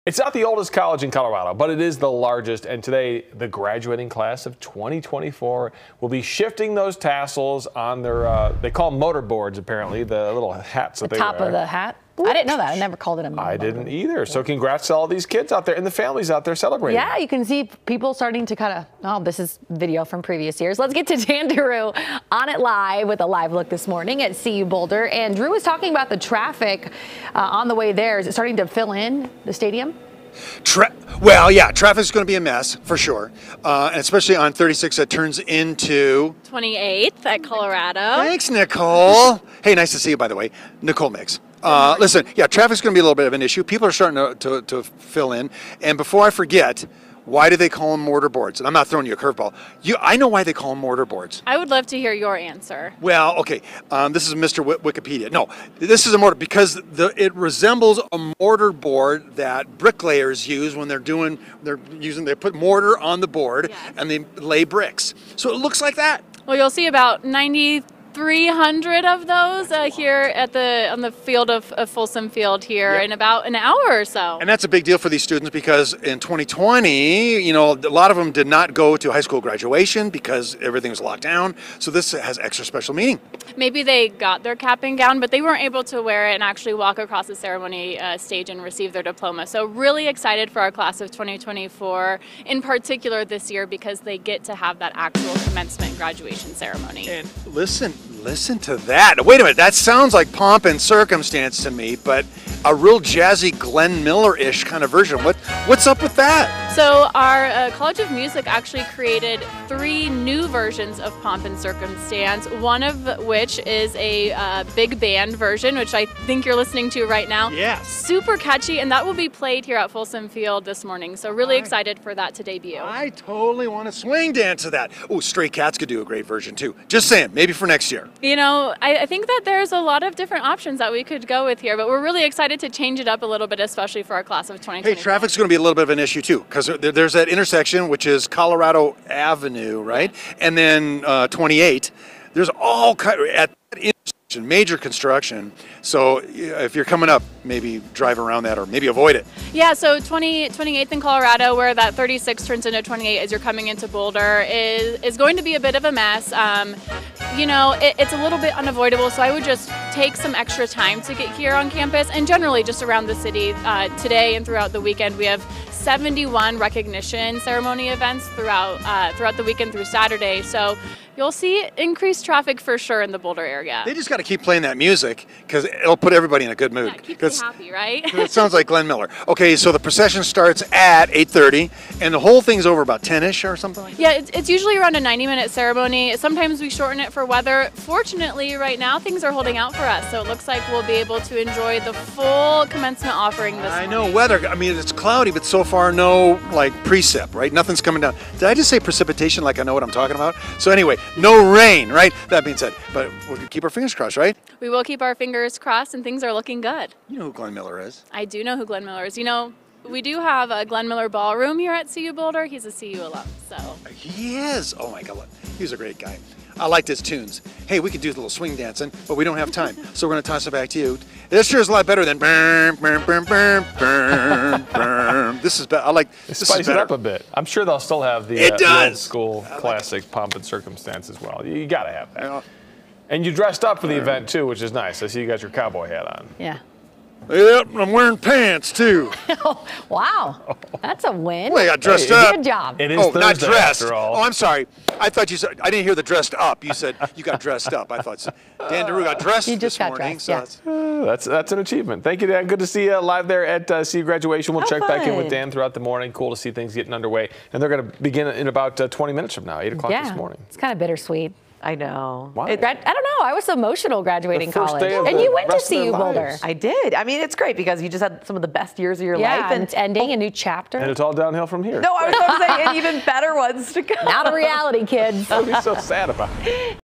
Students prepare to graduate from CU Boulder on Thursday
More than 9,000 students will toss their caps in the air as they celebrate their graduation from the University of Colorado Boulder.